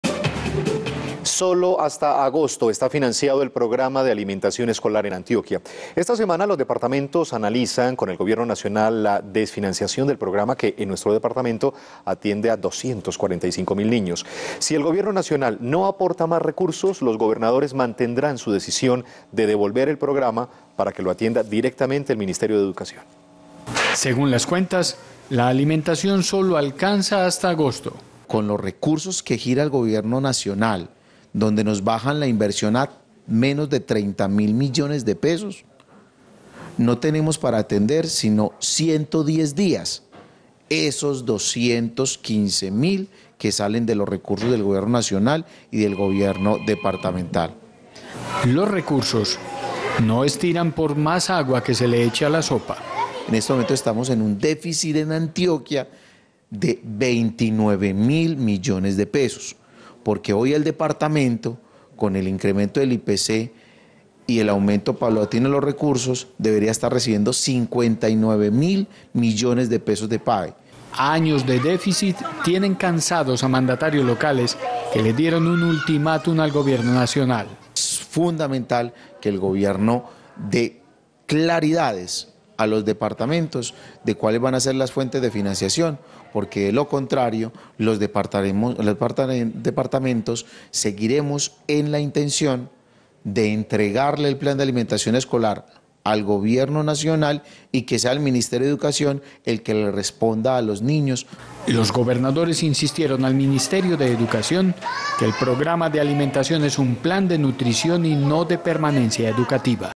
Restaurantes escolares tienen presupuesto solo hasta agosto
Solo hasta agosto está financiado el programa de alimentación escolar en Antioquia.

Esta semana, los departamentos analizan con el Gobierno Nacional la desfinanciación del programa, que en nuestro departamento atiende a 245 mil niños.

Si el Gobierno Nacional no aporta más recursos, los gobernadores mantendrán su decisión de devolver el programa para que lo atienda directamente el Ministerio de Educación.